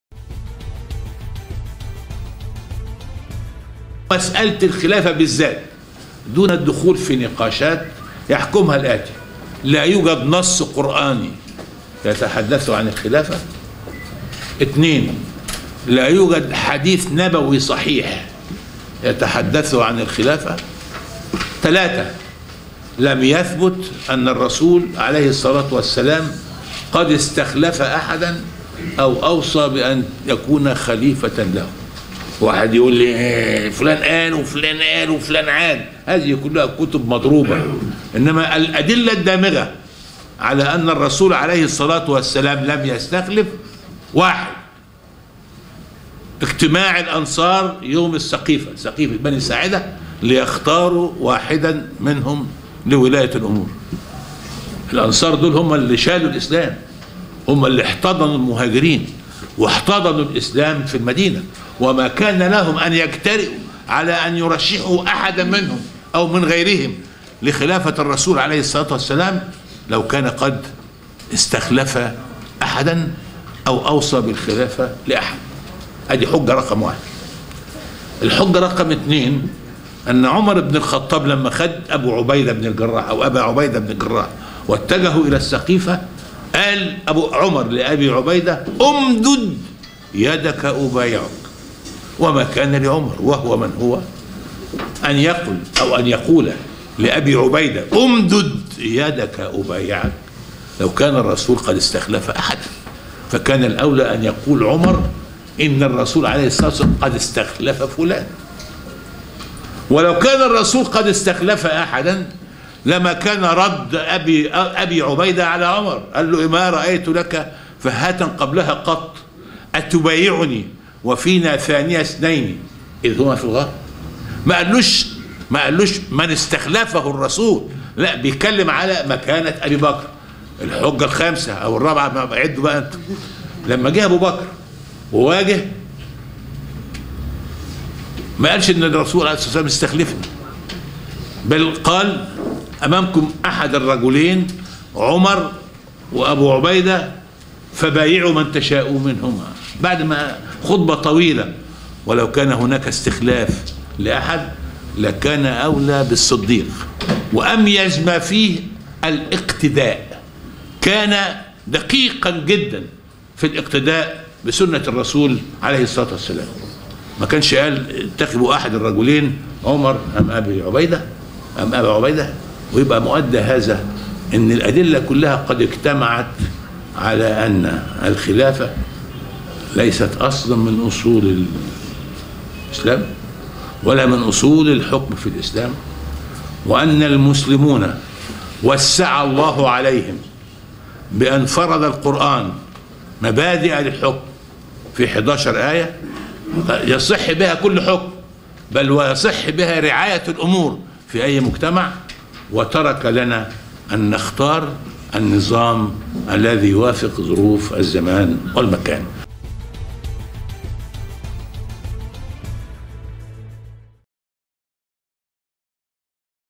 مصر العربية | 5 حجج تُبرهن أن الخلافة ليست من أصول الحكم في الإسلام
حفل توقيع كتاب "دماء على جدار السلطة" للكاتب رجائي عطية، ويقول فيه أن الإسلام بريء من العنف والدماء، أنها وليدة الممارسات السياسية التي امتدت عبر 14 قرن، وأن أخطر ما في أي فكر هو اقترانه بالعقيدة الدينية، يبرهن بالحجج أن الخلافة ليست من أصول الحكم في الإسلام.